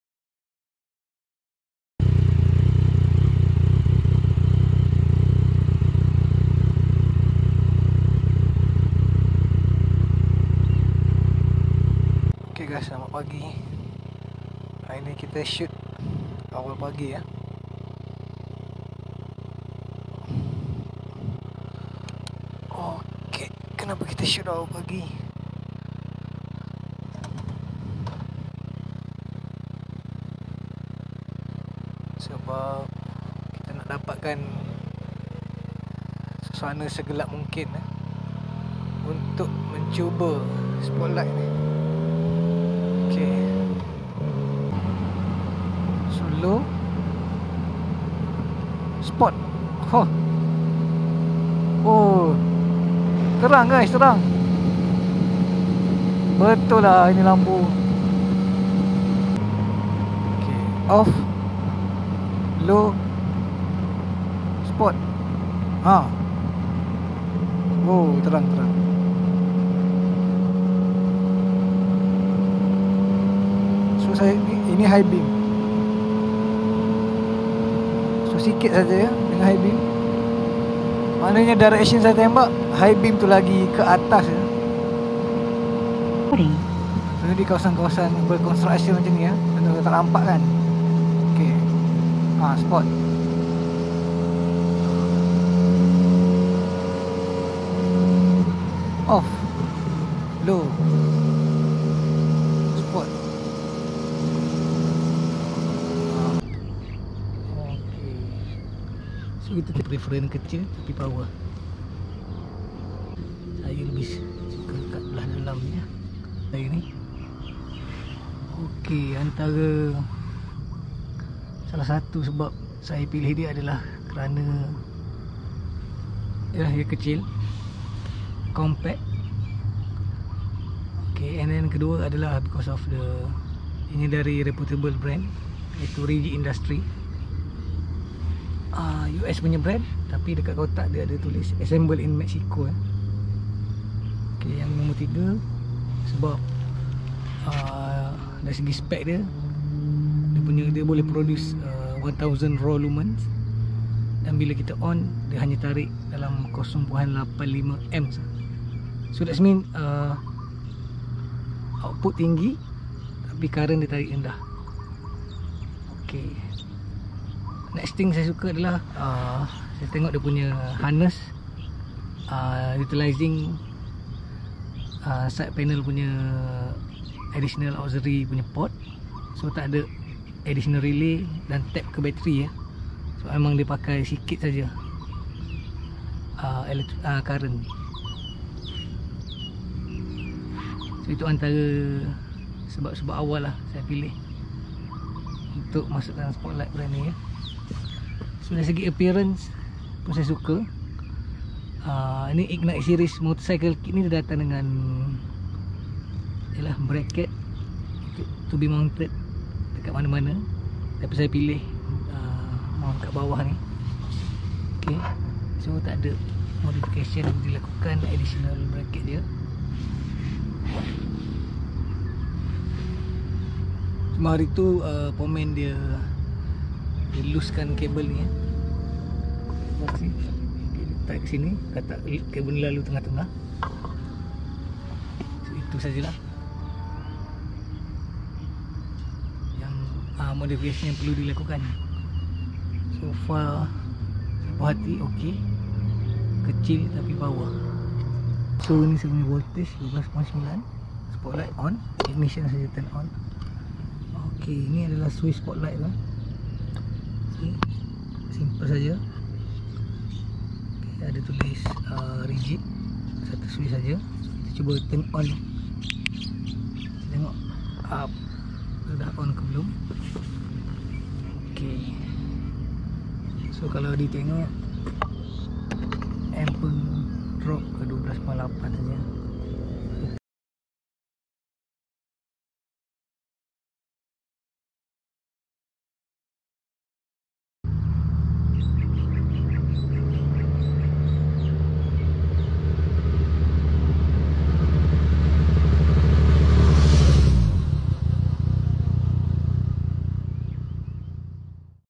My Aux light : Lampu tambahan ku : Rigid Ignite Motorcycle Kit on Yamaha Tracer
1st test and Impression on newly installed Rigid Industries Ignite Motorcycle Kit
Beli n pasang di : Chan Bikers Sekyen 15 Shah Alam..

Harga (dulu) : RM950-1000